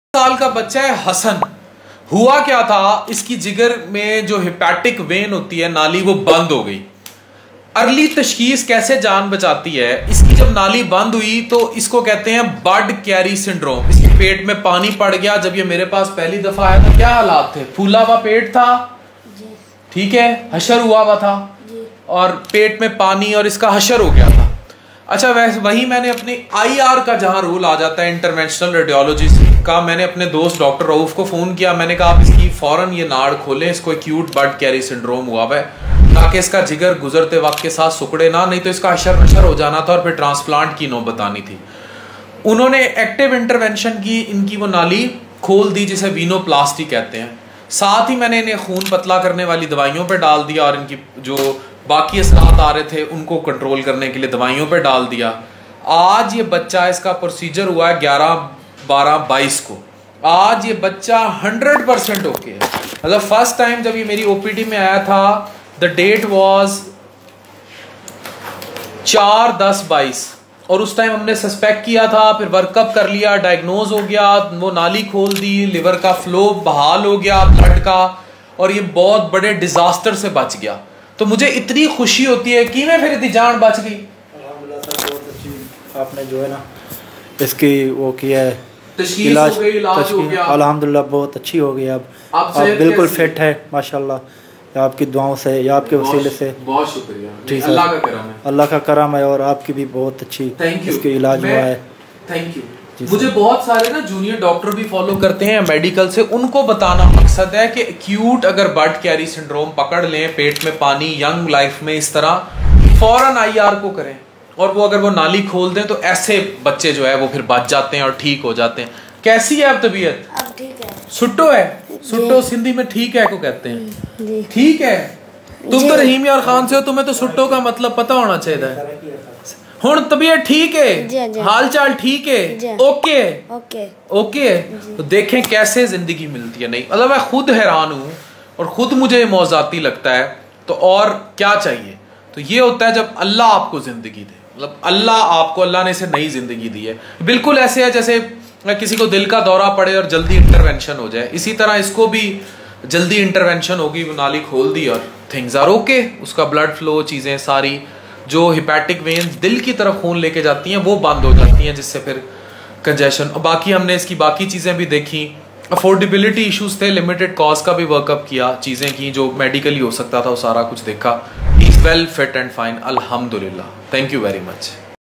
Jigar Ki Khoon Ki Naali Band thi. Waqt Per ilaaj Krny Sy Thek Ho Gai || Dr Affan Qaiser
اس بچے کی جگر کی خون کی نالی بند ہوئی تھی۔ وقت پر علاج سےکھل گئی۔ اللہ نے اس کو دوسری زندگی دے دی۔